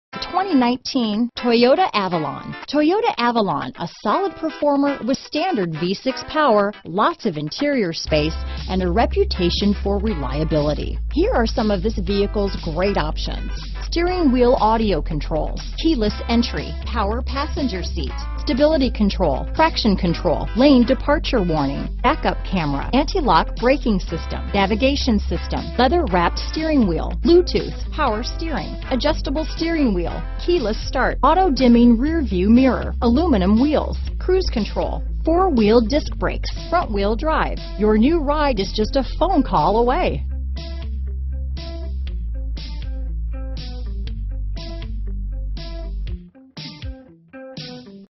2019 Toyota Avalon Naperville, Elmhurst, Lombard, La Grange, Bolingbrook, IL T70085
Wind Chill Pearl New 2019 Toyota Avalon available in Naperville, Illinois at Oakbrook Toyota. Servicing the Elmhurst, Lombard, La Grange, Bolingbrook, IL area.

Oakbrook Toyota
550 E Ogden Ave

Carpet Mat PackageAdvanced Safety Package Includes: Intelligent Clearance Sonor, Bird's Eye View Camera w/Perimeter Scan and Rear Cross-Traffic Alert w/BrakingSpecial Color